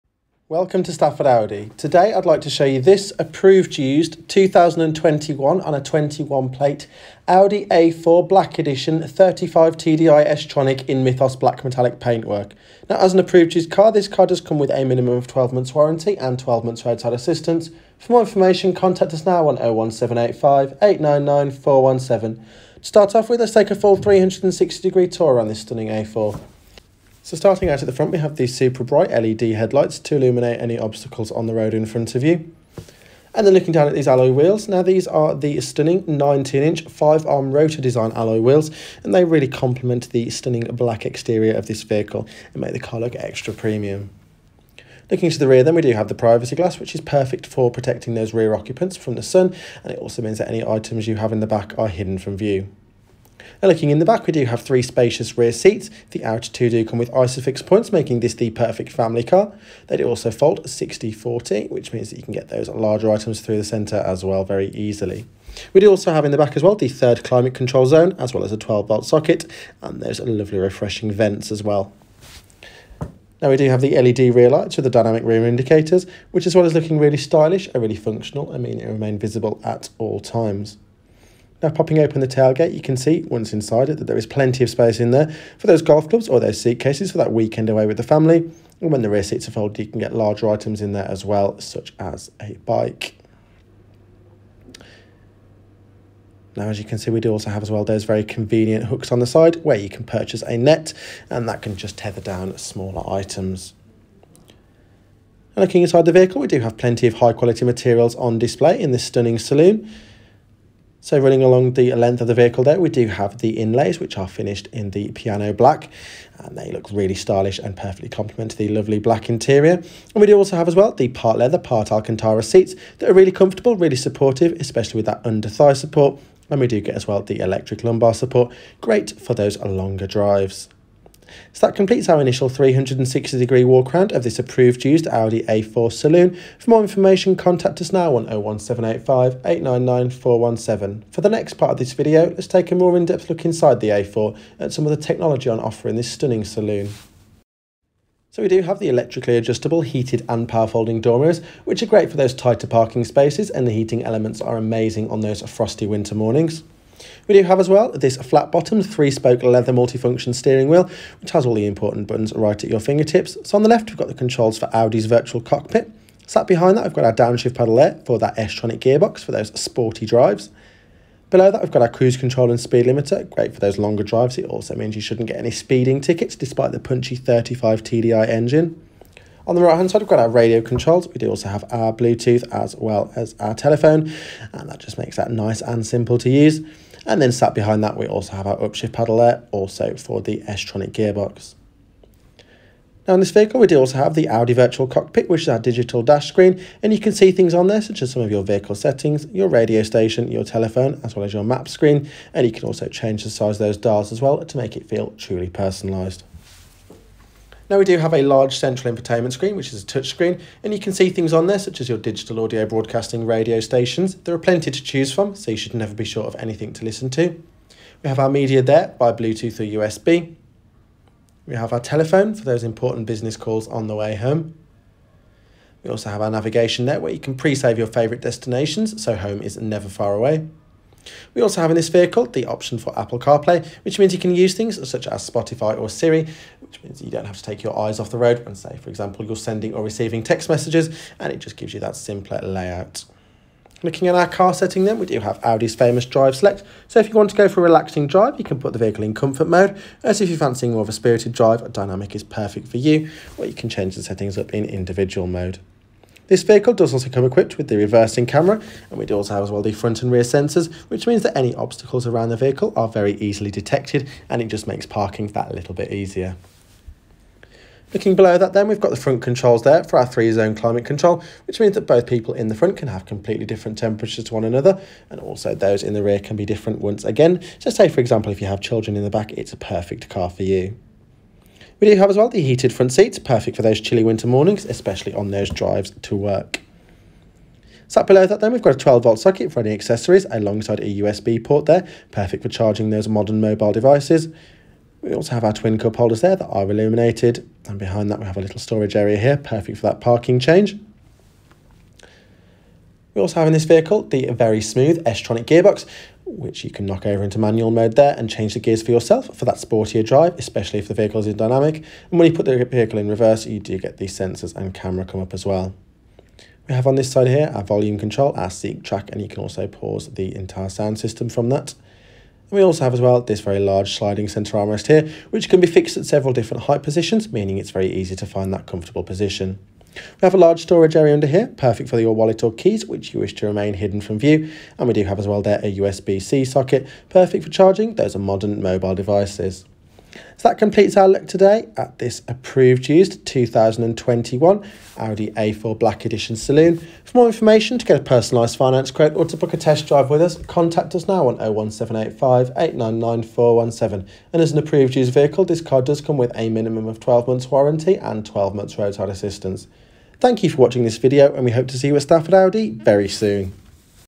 Approved Used Audi A4 Black Edition 35 TDI 163 PS S Tronic at Stafford Audi
Now in stock at Stafford Audi, this Approved Used Audi A4 Black Edition 35 TDI 163 PS S Tronic in Mythos Black Metallic.

Registration number:
Fuel: Diesel
Transmission: Automatic
Mileage:

Features:
19in Alloy Wheels
Flat-Bottomed 3-Spoke Leather High Multi-Function Steering Wheel
Front & Rear Parking Sensors
Heated Front Sport seats
LED headlights
Privacy Glass
Reversing Camera

00:00 - Introduction
00:20 - Exterior
00:54 - Rear Seats
01:22 - Boot Space
02:28 - Interior Tour
05:45 - Contact Information